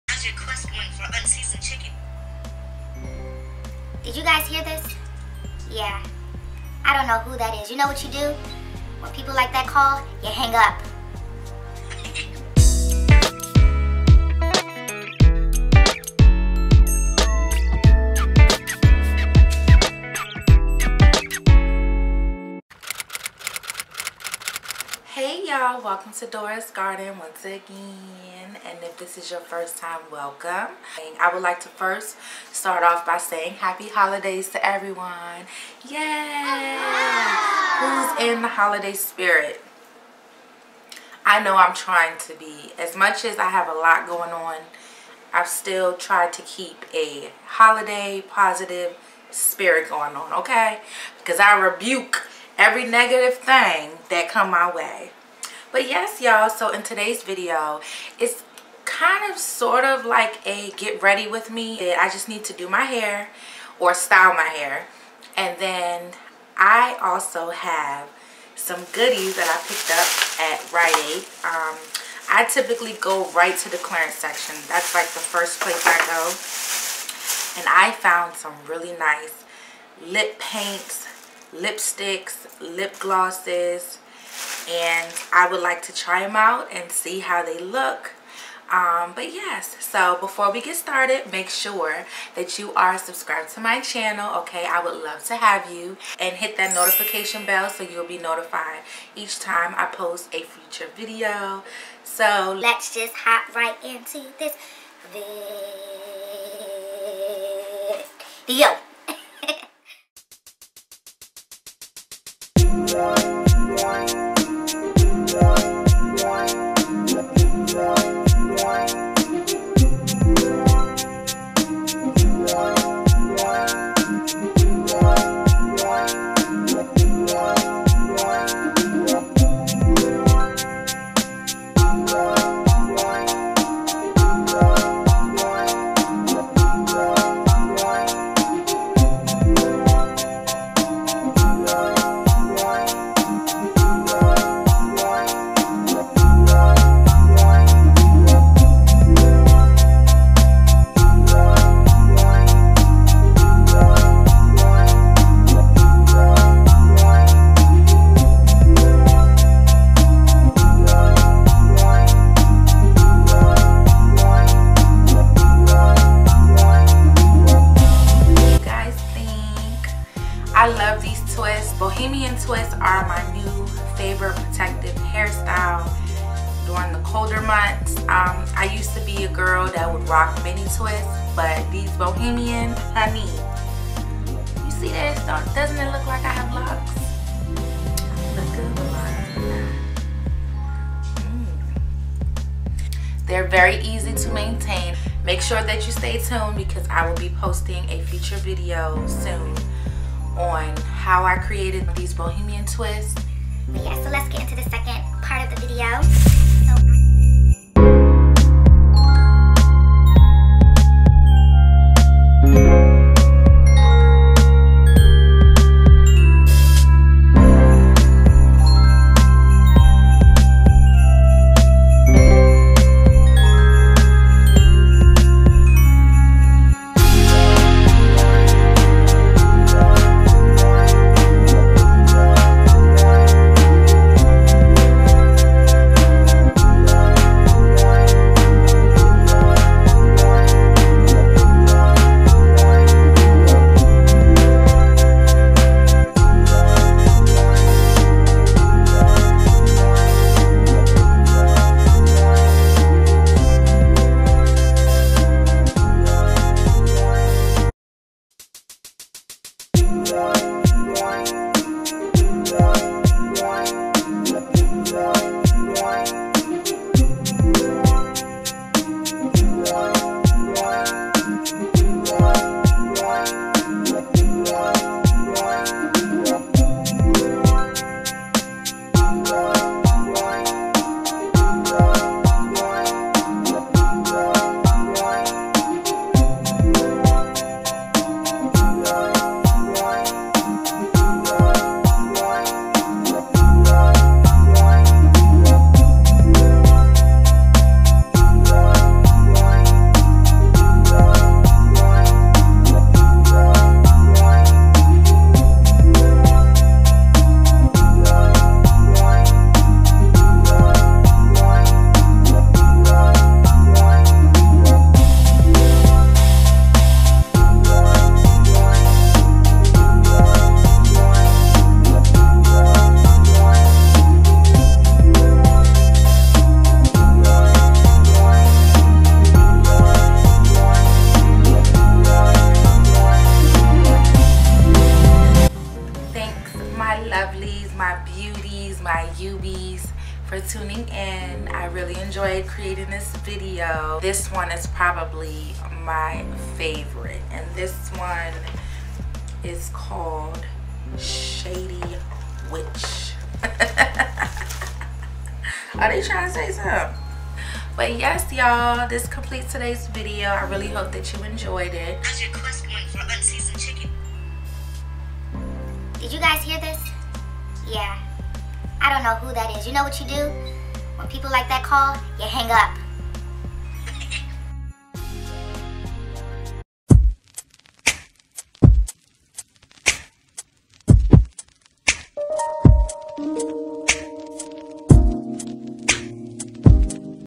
Quick Holiday Hair & Lippies💄
Simple yet, very cute hair and liippies under 5 mins and for an inexpensive price!